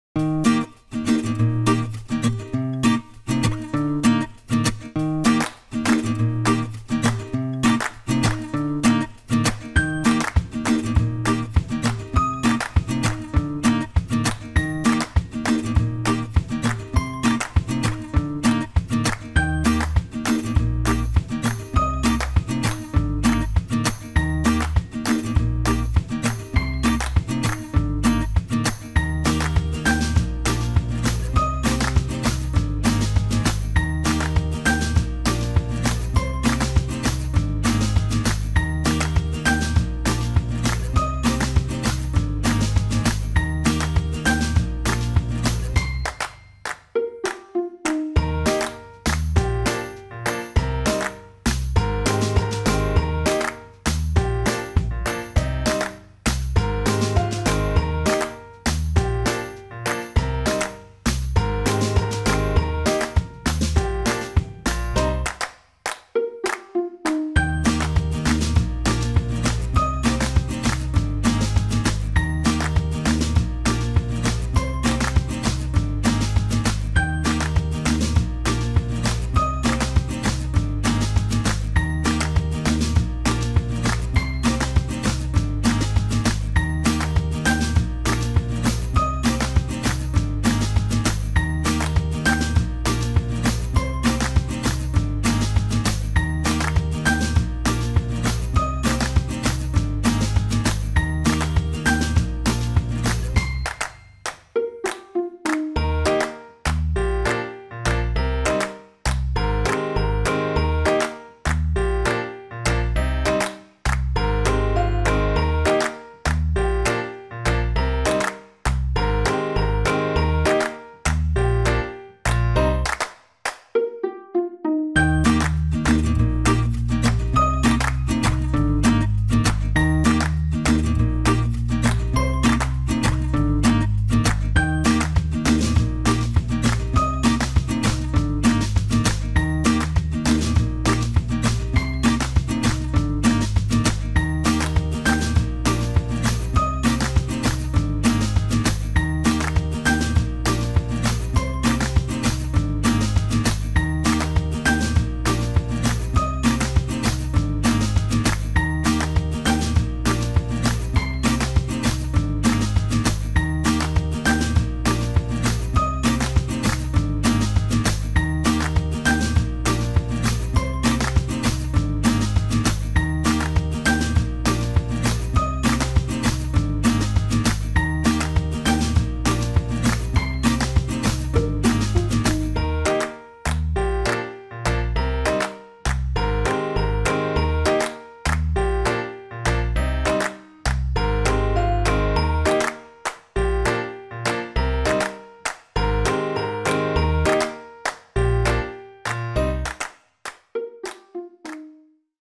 Paper Crafts for Kids - Easy Bat Halloween Craft for Kids
Nothing beats simple when it comes to crafting – we just know you are going to love this Paper Crafts for Kids - Easy Bat Halloween Craft for Kids. This one makes a great Halloween craft for kids to make and it can be easily made in the classroom – frugal, quick and fun. Plus you can hang them and make cool classroom decor.